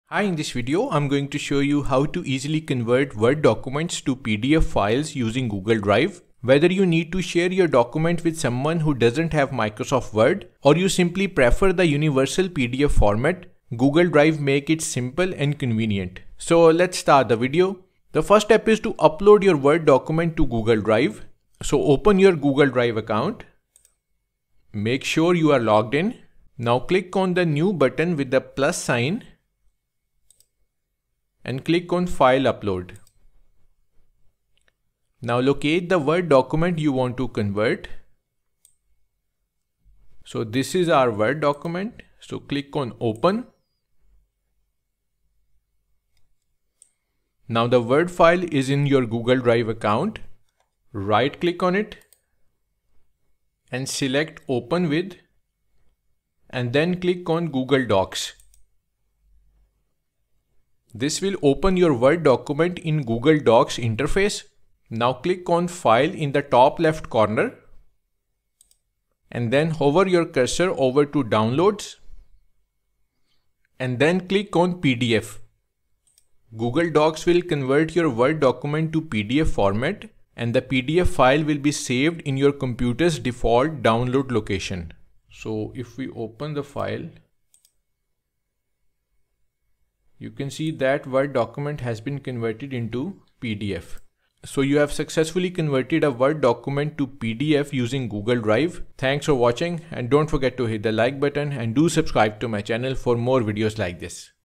How to Convert Word to PDF using Google Drive 2024
This video guides about how to convert word to pdf using google drive.

To learn how to convert word to pdf in google drive, follow the step-by-step guide.

Converting Word to PDF using Google Drive ensures easy accessibility, sharing, and collaboration on your documents from anywhere with an internet connection.

Here are the Steps to Convert Word Document to a PDF on Google Drive:

1. Open Google Drive and make sure your are logged in to your google account.

2. Click on the "New" button with "+" in the top left corner of the screen.

3. Select "File upload" from the dropdown menu.

4. Navigate to the Word Document you want to convert tp PDF.

5. Once upload is complete, right click on Word Document (.docx file) and select "Open with" and click on "Google Docs".

6. In Google Docs Interface, click on "File" and hover your cursor to download and then click on PDF.

Your Word document will be converted to PDF format and the PDF file will be saved in your computer's default download location.

So that's how to convert how to convert word to pdf using Google Drive.